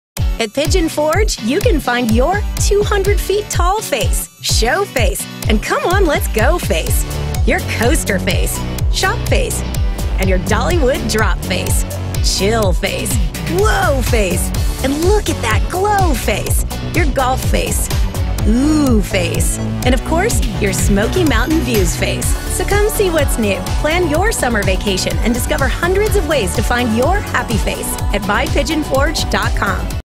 Find Your Happy Face in Pigeon Forge
Visit Pigeon Forge, Tennessee to find your Happy Face. Your Show Face. Your Coast Face. And your Smoky Mountain Views Face. Come see what's new!